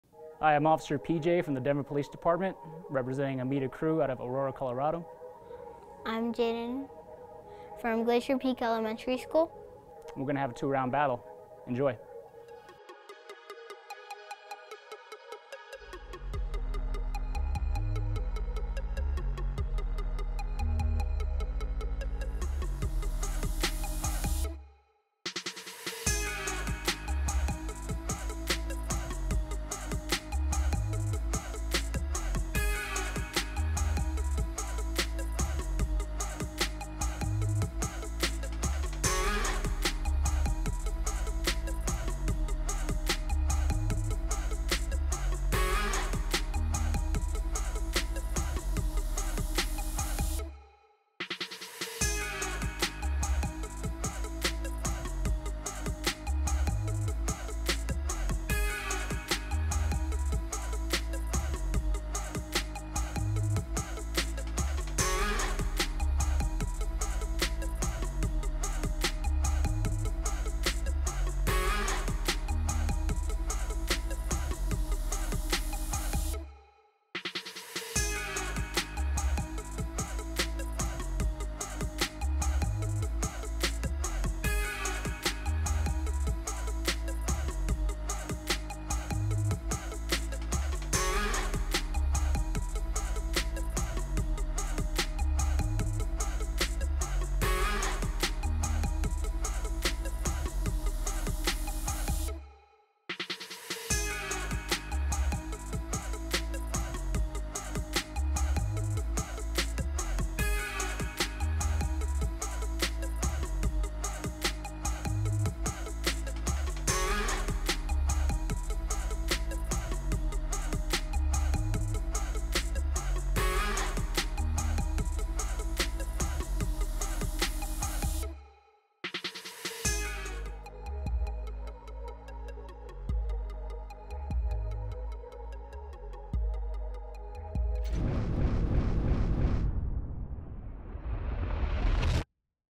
Breakin' with PJ and Jayden
You've seen their battle on the 16th Street Mall - now watch as DPD Officer Thanarat "PJ" Phuvapaisalkij and 7-year-old Jayden go for round two as they prepare to appear on the Kelly Clarkson Show!